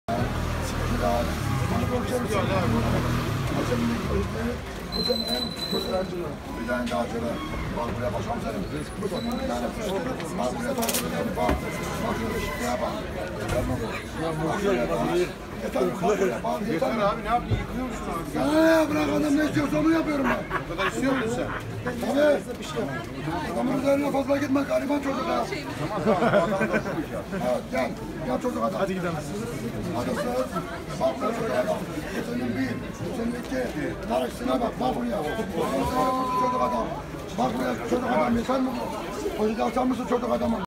Çiğ Köfteci Ali Usta Adamın Üzerine Fazla Gitme Gariban Çocuk
Meşhur çiğ köfteci ali usta limonu çok sıkınca adamın biri ne yapıyorsun yıkıyor musun diyor,ali usta sanane ya adam ne istiyorsa onuyorum diyor